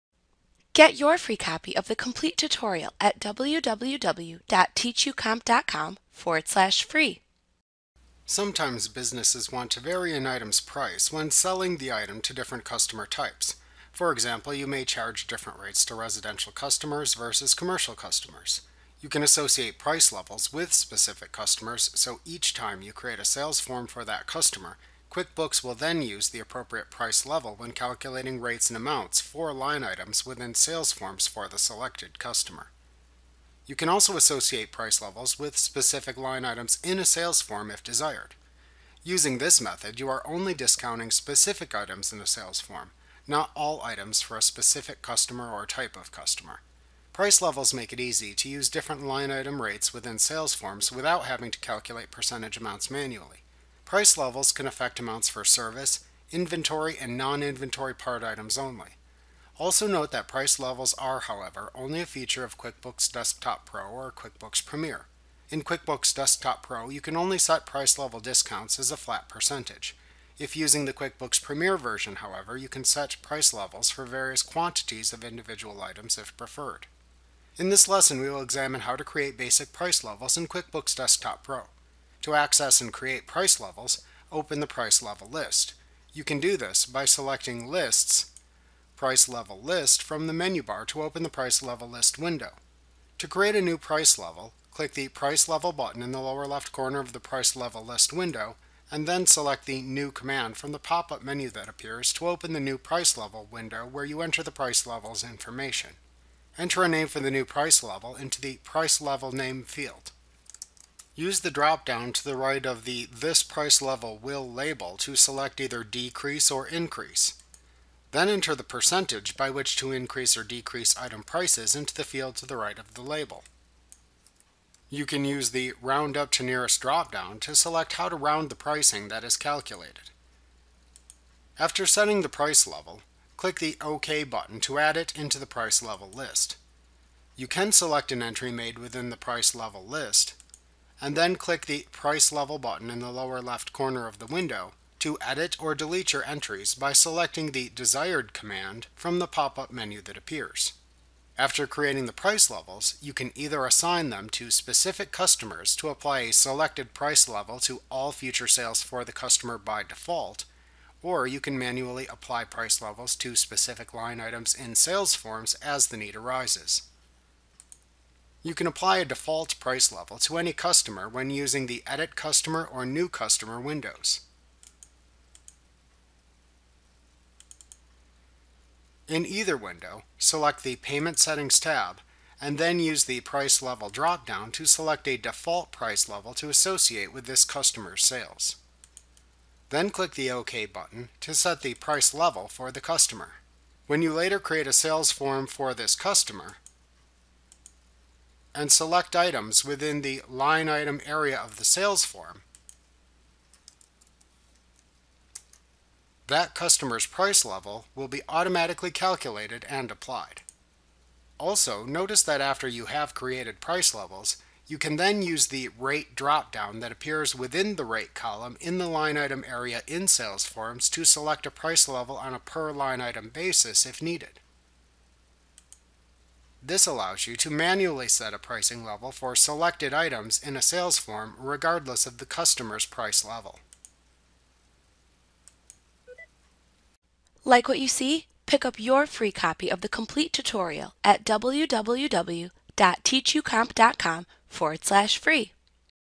Learn How to Create and Use Price Levels in Intuit QuickBooks Desktop Pro 2023: A Training Tutorial
This QuickBooks tutorial video shows you how to create price levels and use price levels in QuickBooks 2023.

This QuickBooks Desktop tutorial video helps you learn how to create price levels and use price levels in QuickBooks Desktop 2023. It also shows how to set a default price level in QuickBooks for a customer and change the price level in a sales form, if needed. This video is from our complete QuickBooks training, _Mastering QuickBooks Made Easy_ ™. *To get the full QuickBooks training, please visit:*